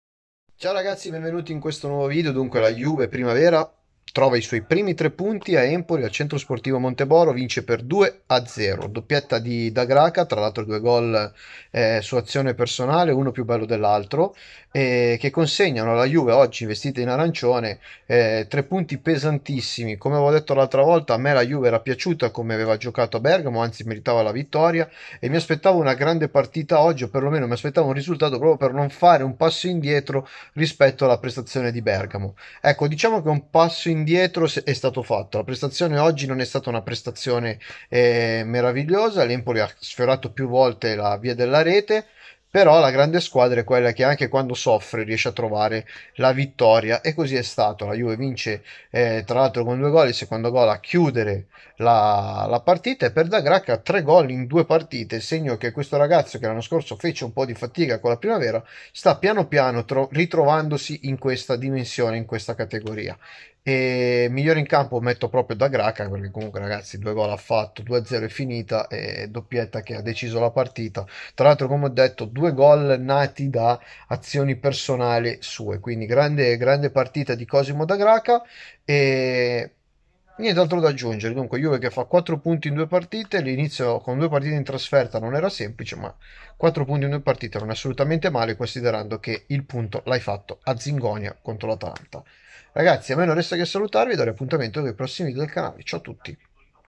Empoli - Juventus 0-2 Campionato Primavera 1
Grande vittoria dei bianconeri al centro sportivo Monteboro